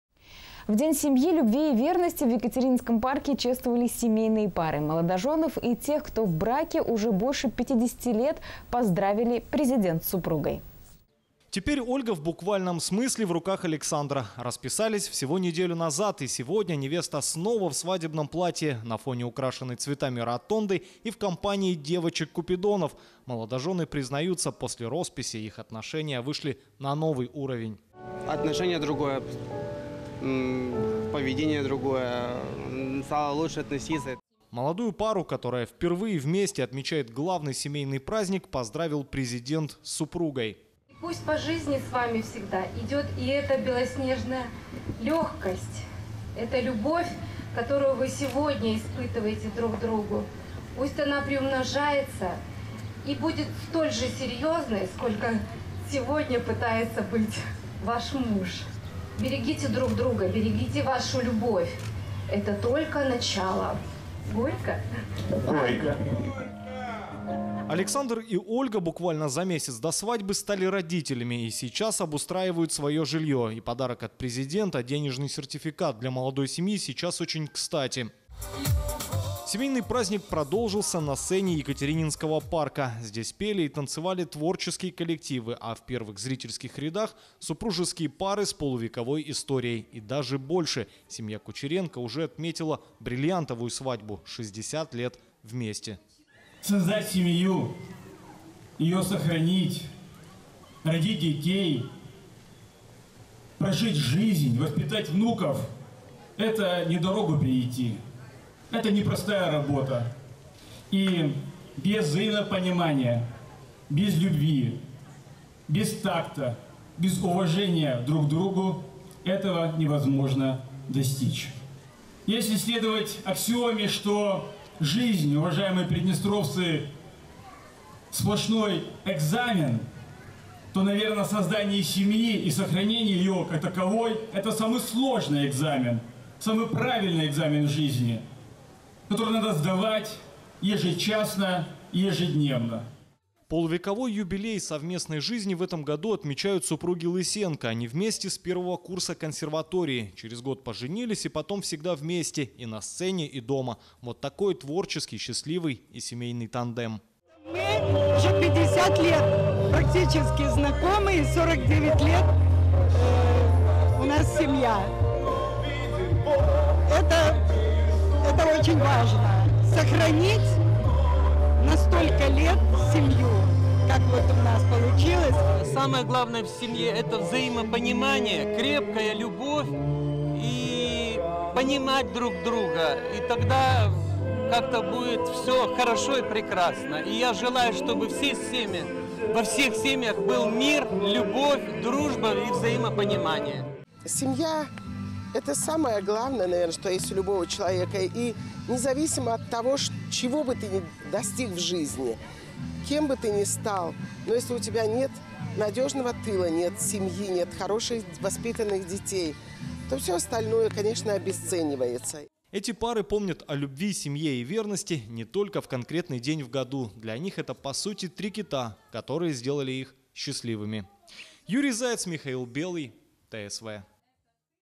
Вместе навсегда
В день семьи любви и верности в Екатерининском парке чествовали семейные пары. Молодожёнов и тех, кто в браке уже больше 50, лет поздравили Президент с супругой.